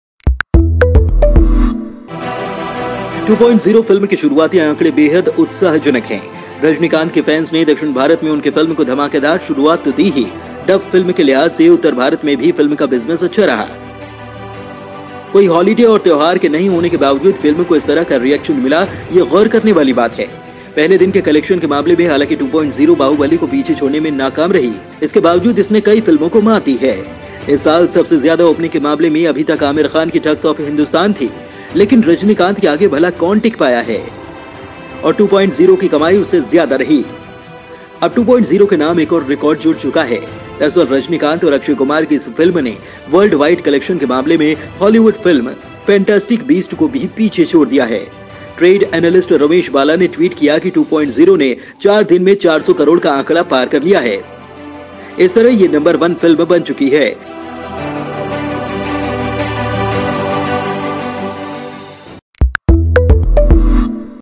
2.0 ने Hollywood की फिल्म को दी दे दी मात, 4 दिन में ही कलेक्शन इतने करोड़
2.0 The initial figures of the movie are very encouraging. The fiance of Rajinikanth gave his film a big start in South India, in the direction of the film, in the direction of the film, the film also had good business in North India. Regardless of the holidays and festivals, the film got such a reaction. It is worth considering. In the case of first day collection, however, 2.0 failed to beat Bahubali, despite this it has defeated many films. So far this year, in the case of the highest openings, Aamir Khan had the Thugs of Hindostan, but who has remained ahead of Rajinikanth and the earning of 2.0 is more than that. Now another record has been added to 2.0. Indeed, this film of Rajinikanth and Akshay Kumar has outstripped the Hollywood film Fantastic Beast in the case of World Wide Collection. Trade analyst Ramesh Bala tweeted that 2.0 has crossed the 400 million mark in 4 days. This has become the number one movie in this way. While Fantastic Beast was at number two, it earned World Wide 360 ​​million, and was expected to increase in Weekend 2.0 in the weekend than the first day. According to Ramesh Bala, "Initial data of 2.0, on Sunday, 2.0 has done business of about 32 crore." On the first day, where the film earned Rs 20.25 crore in Hindi version, on the second day i.e. on Friday it has earned 19 crore ...